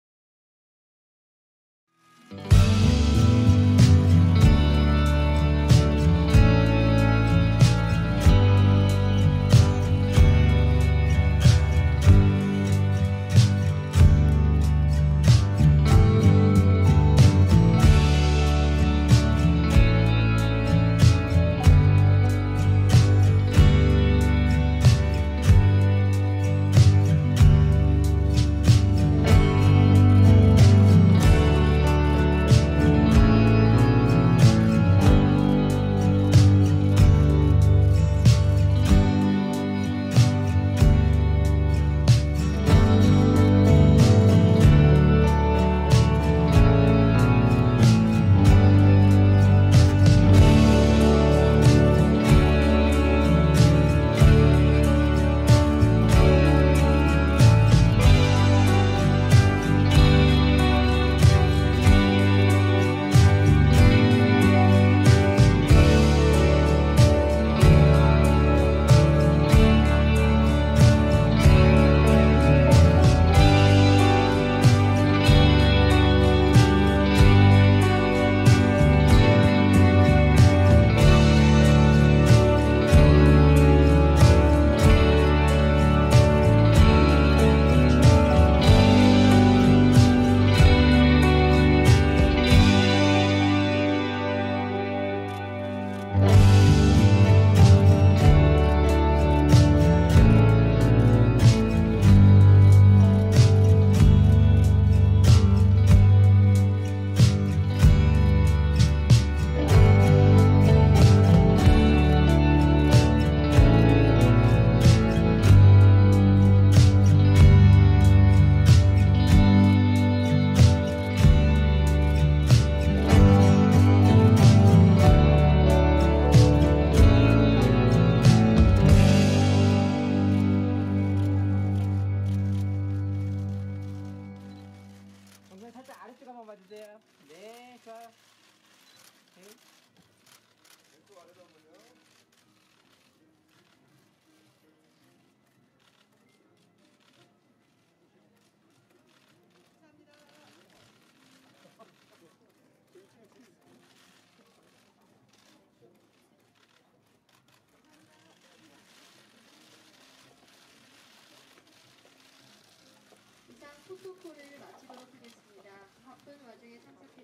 190507 이영애 아직도 산소같은 여자, 더 히스토리 오브 후 '2019 왕후의 비밀展'
7일 오후 서울시 성북구 대사관로 한국가구박물관에서 LG생활건강 궁중 럭셔리 브랜드 '더 히스토리 오브 후'가 브랜드 모델 배우 이영애와 함께 '왕후의 비밀展' 포토 이벤트가 열려 이영애가 포즈를 취하고 있다.